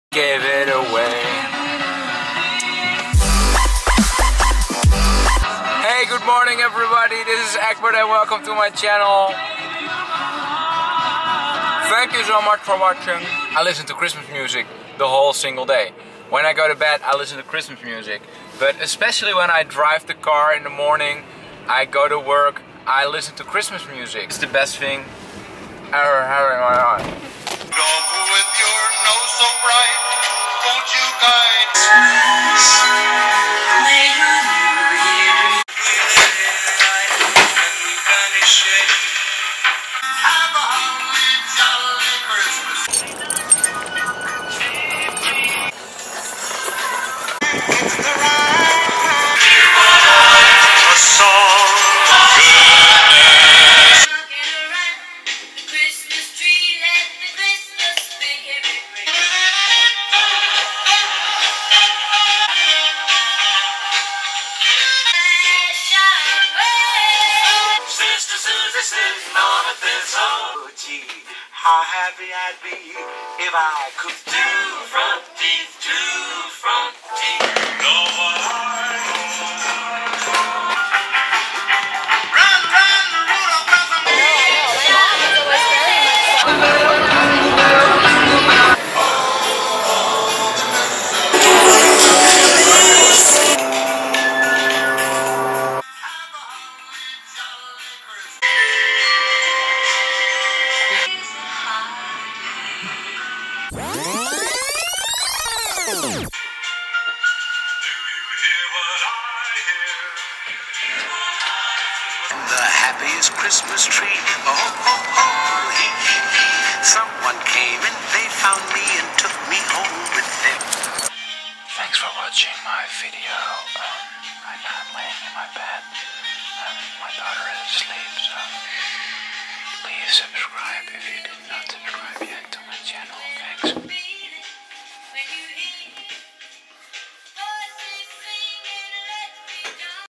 MY GUILTY PLEASURE | OhOhMerica
One of my guilty pleasures is to listen Christmas music 24/7. Not only around Christmas, but every single day of the year. Not only in the car or in my office. No I listen to it everywhere. I listen Christmas music when I wake up, I listen Christmas music when have breakfast, I listen Christmas music when I walk the dog, I listen Christmas music when I watch an American Football game, I listen Christmas music when I eat, I listen Christmas music when I play with my newborn. I just listen to Christmas music every single day.

Right now I traveled to the Netherlands to celebrate Christmas with my family. I can tell you one thing, Oh boy I listened to Christmas music a lot in that airplane, in the car to my mother in law.

We will celebrate first christmas day in Oldenzaal with my parents and second christmas day in utrecht with my family in law. We all love Christmas music and will listen to it 24/7.